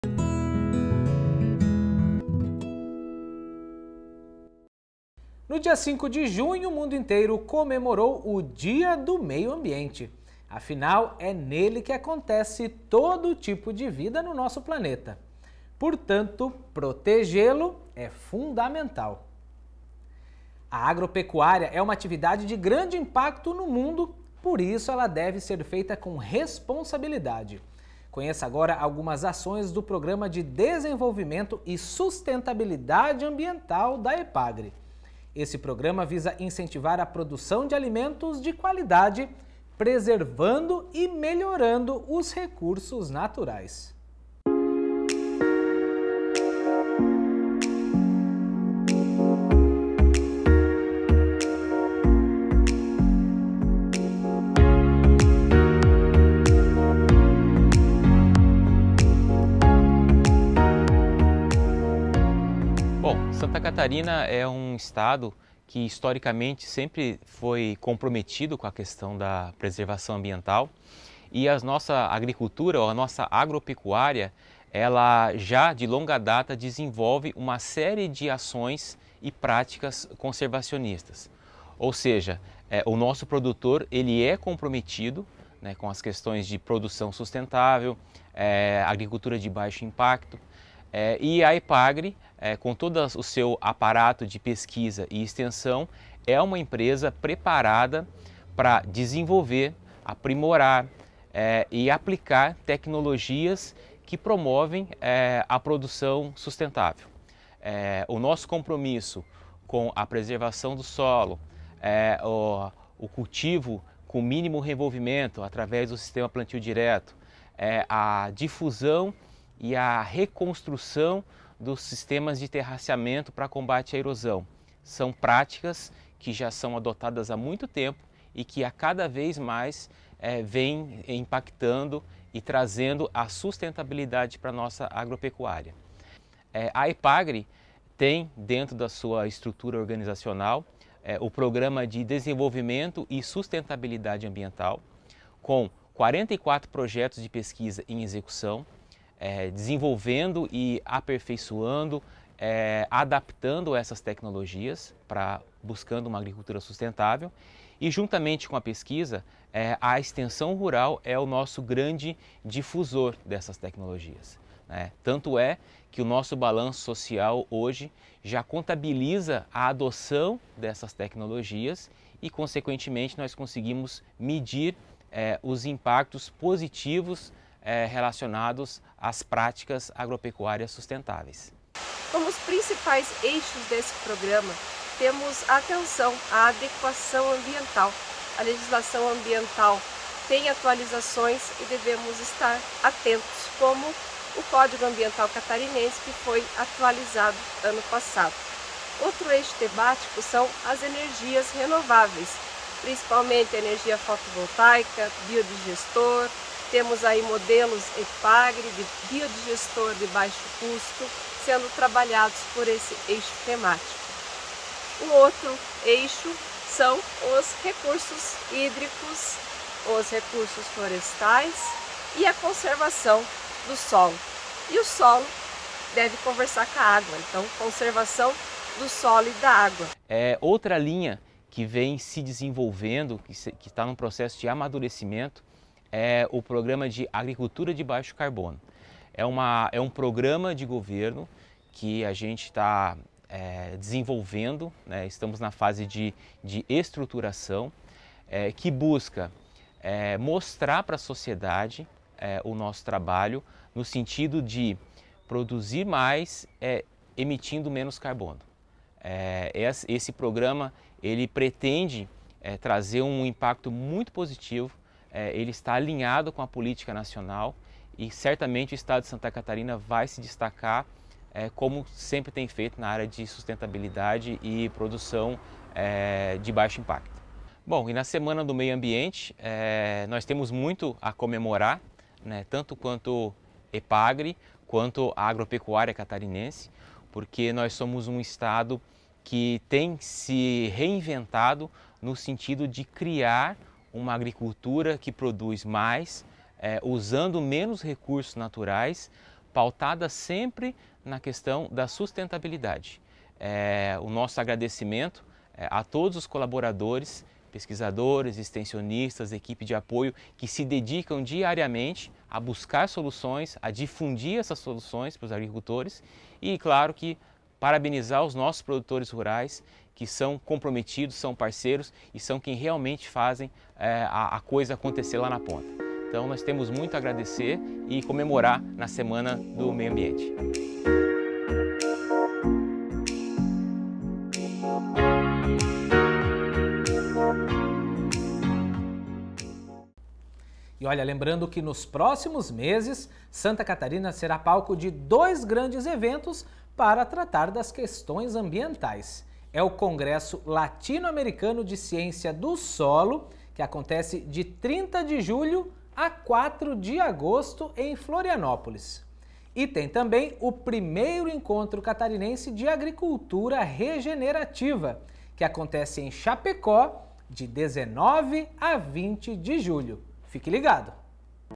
Semana do Meio Ambiente na Epagri: conheça algumas ações ambientais da empresa
Nessa semana do meio ambiente, o SC Agricultura dá destaque ao Programa de Desenvolvimento e Sustentabilidade Ambiental da Epagri. As ações do Programa estimulam a produção de alimentos seguros, nutritivos e economicamente viáveis com o mínimo impacto ao meio ambiente.

As práticas de terraceamento, plantio direto, revolvimento mínimo do solo, proteção de recursos hídricos e biodigestores são alguns exemplos das ações executadas pelo Programa.